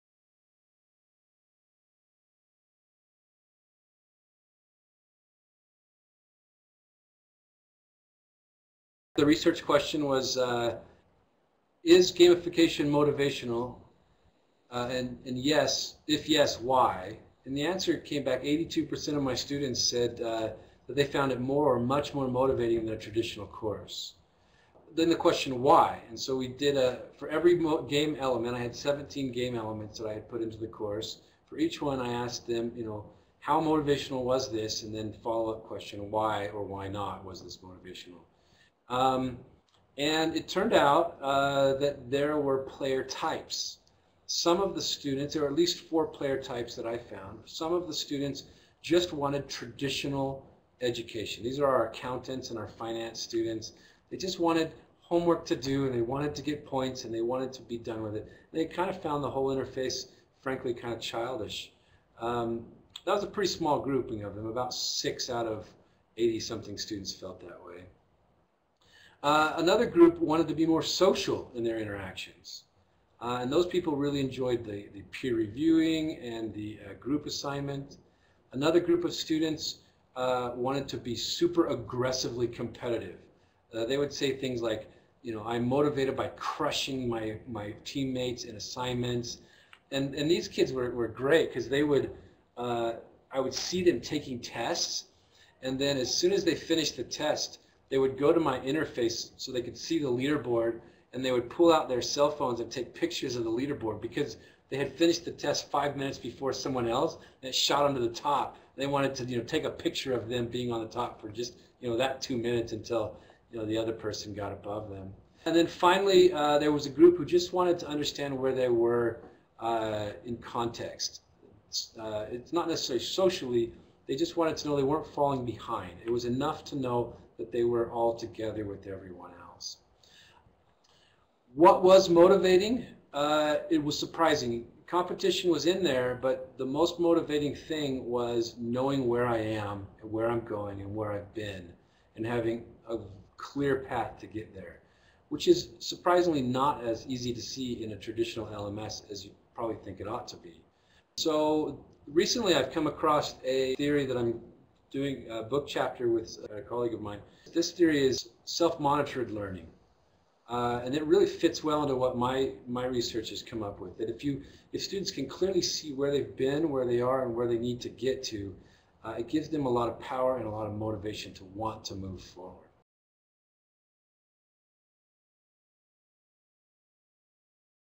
Amely Interview 2 - Research lessons from Project Delphinium
This is the second in a series of interview segments conducted by Janell Amely, an Instructional Technology student at USU. This interview of Dr. Jared R Chapman describes what we have learned from Project Delphinium about how and why educational gamification works. to follow our progress!